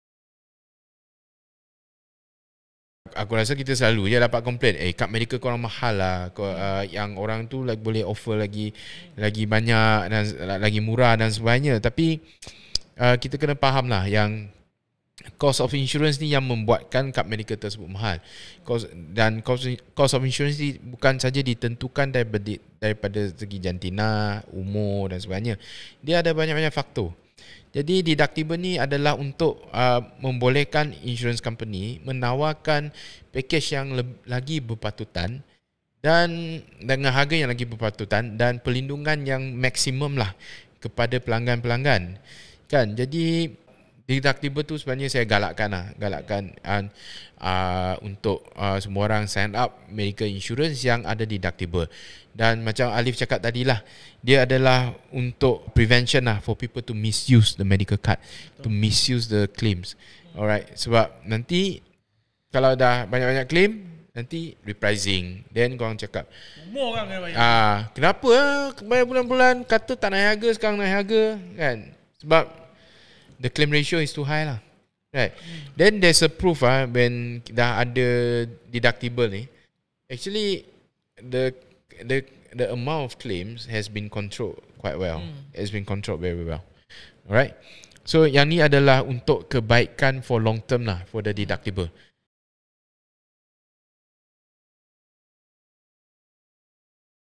Kad medical: Kebaikan tentang deductible
Deductible membantu mengawal jumlah tuntutan yang dibuat oleh pelanggan. Dengan adanya deductible, pelanggan akan lebih berhati-hati dalam membuat tuntutan, hanya menggunakan kad perubatan untuk kes yang benar-benar perlu.